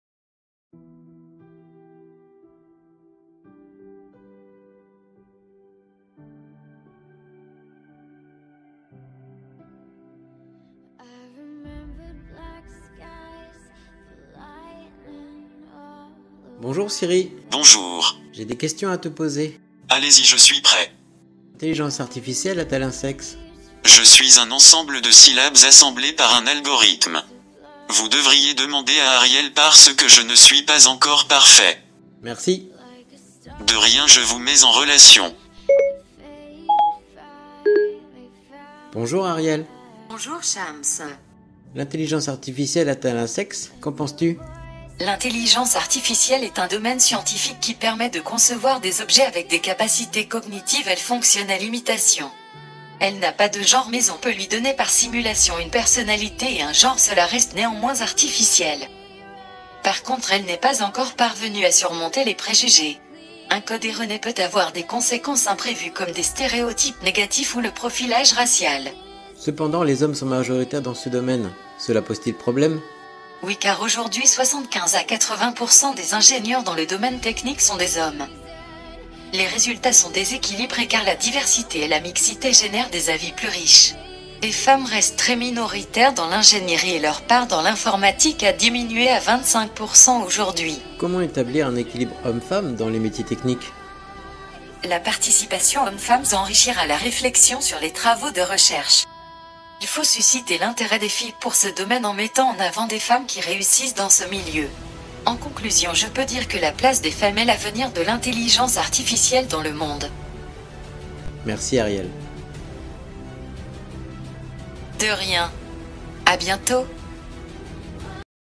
IA ARIEL
Vidéo créée pour le challenge Atos. Lors du challenge, j'ai recueilli 495 votes sur 1600 et plus de 1000 vues en une semaine.
AR-tific-IEL réponds à vos questions !